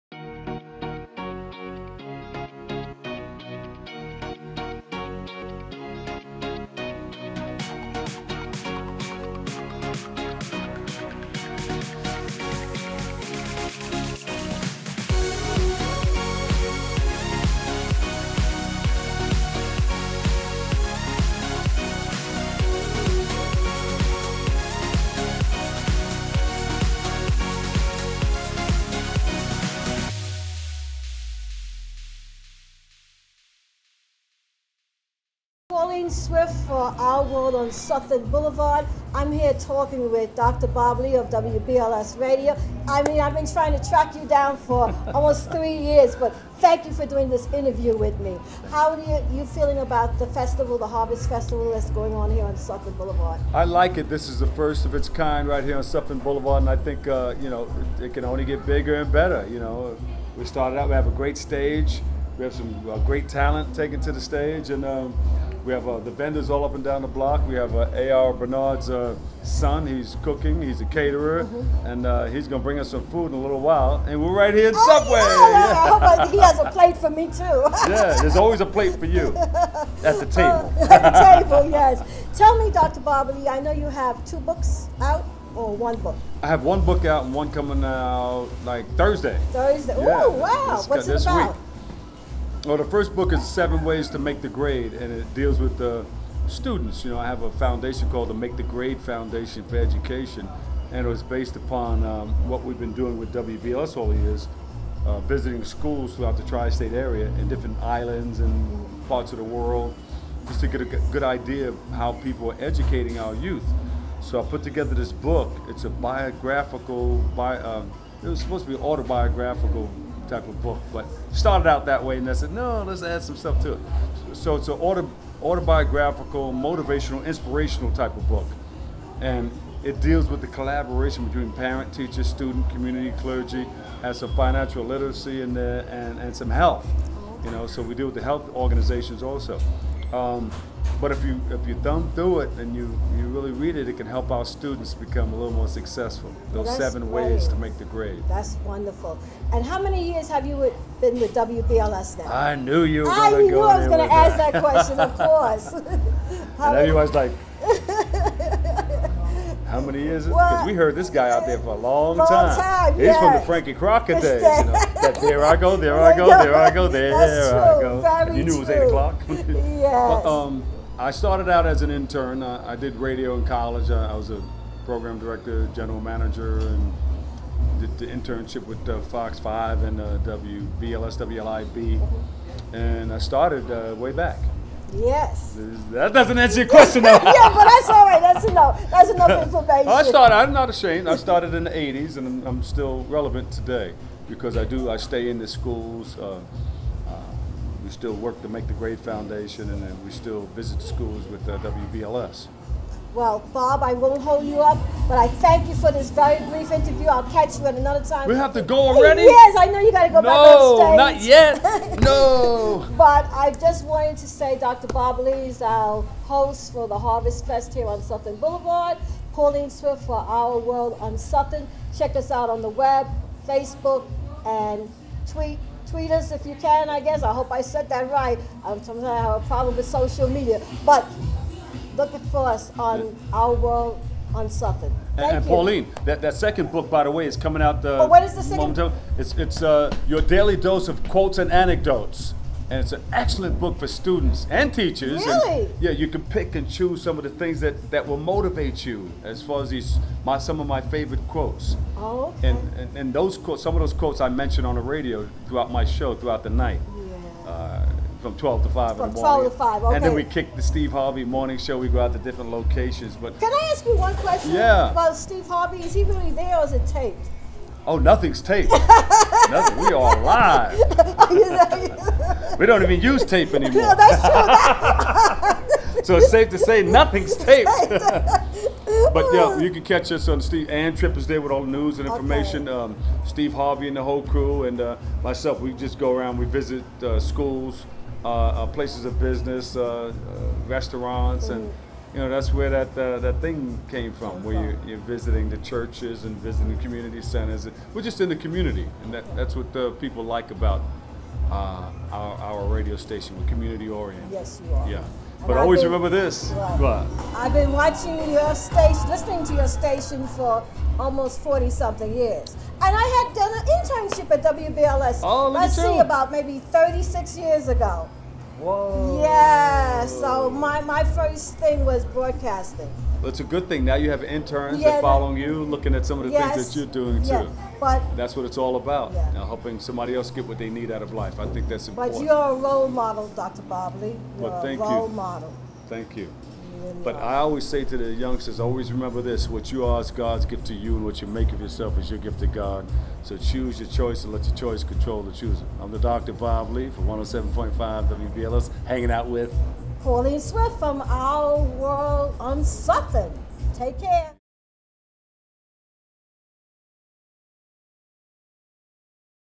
Dr.Bob Lee Interview at the Sutphin Blvd Harvest Festival
Our World On Sutphin News Interviews Dr.Bob Lee from from WBLS.
In addition to his on-air roles, Lee also is the Community Affairs Director for WBLS. As part of his community-based work, Lee has developed strong ties to many local and national politicians and public figures, including President Clinton, Mayor Michael Bloomberg, Mayor David Dinkins, and Bronx Borough President Adolfo Carrión.